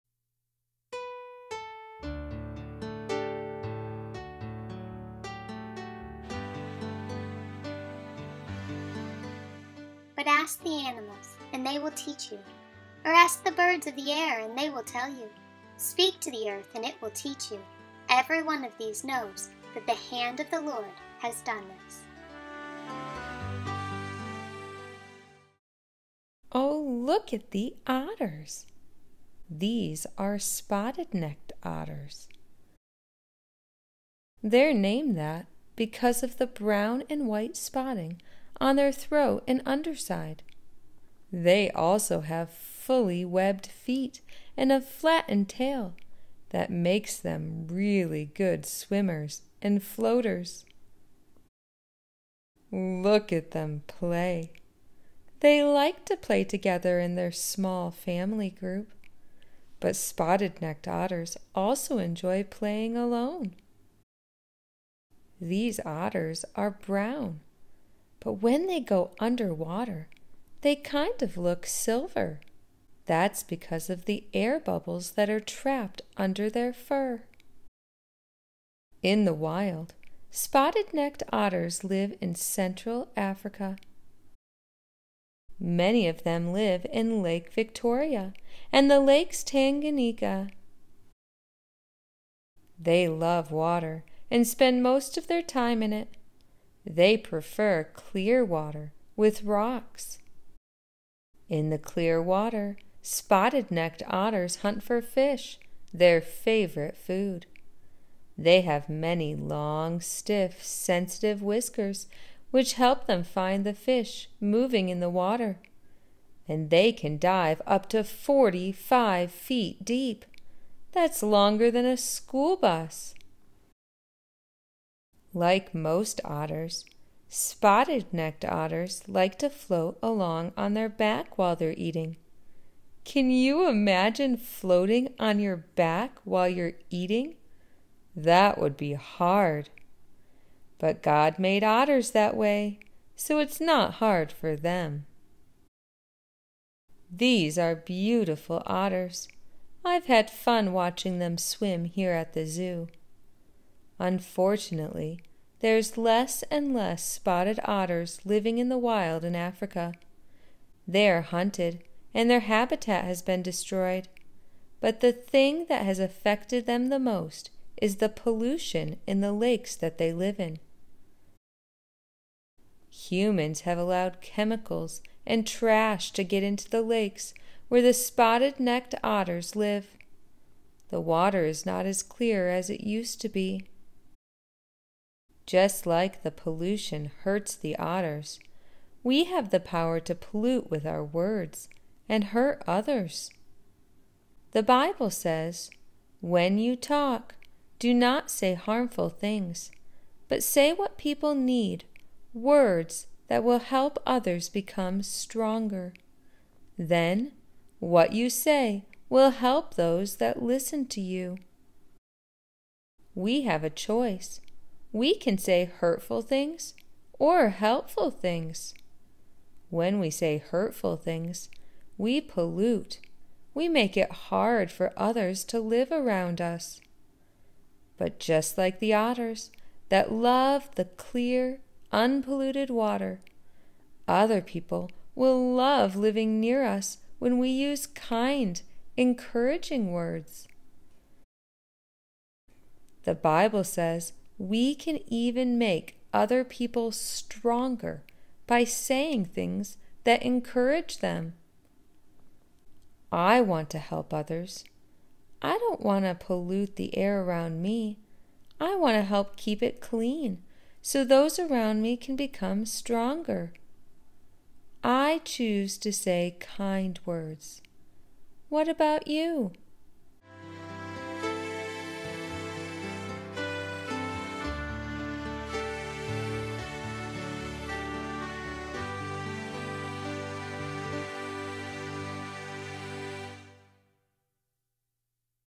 Pollution Hurts the Otters - God's World
Just like pollution hurts spotted-necked otters, we have the power to pollute with our words and hurt others.
The Bible says, “When you talk, do not say harmful things. But say what people need—words that will help others become stronger. Then what you say will help those who listen to you.” (Ephesians 4:29)
Let's learn from God's World!

Photo Credits:
Amanda Hasty
(Some images are in the public domain)